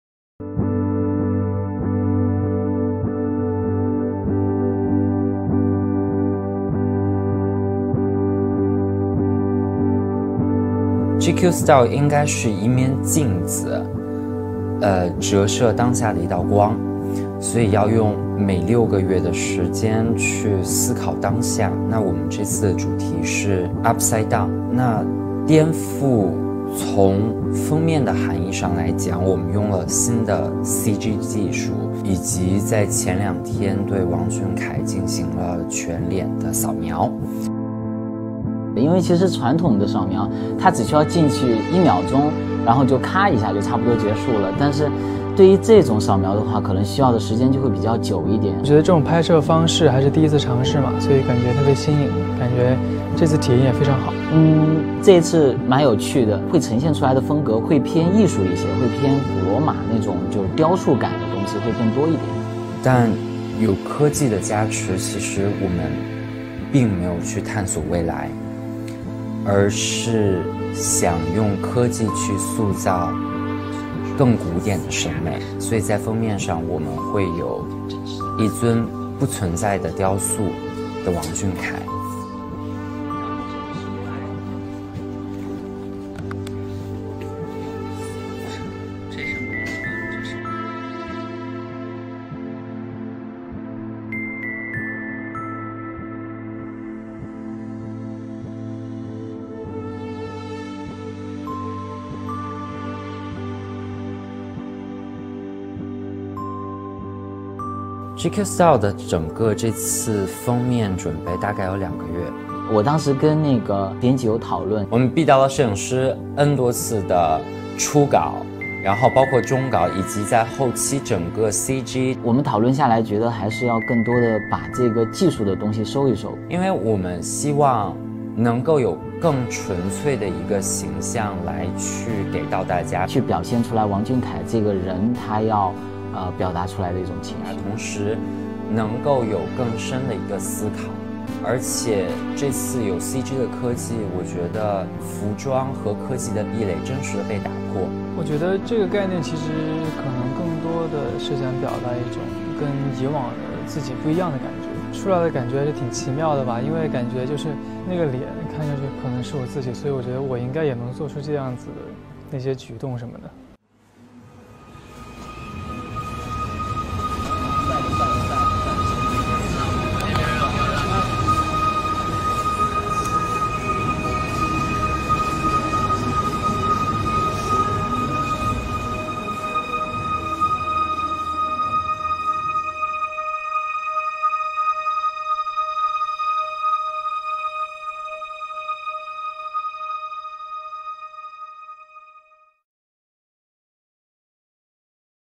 [Vietsub] Vương Tuấn Khải: Hậu trường chụp ảnh tạp chí GQ Style số Thu Đông - UP SIDE DOWN
Cre: 智族GQ
💙Translators: Ying, JSC
💙Timer: Di
💙Typeset + Encode: Meomeo
DO NOT RE-UP. JUST SHAR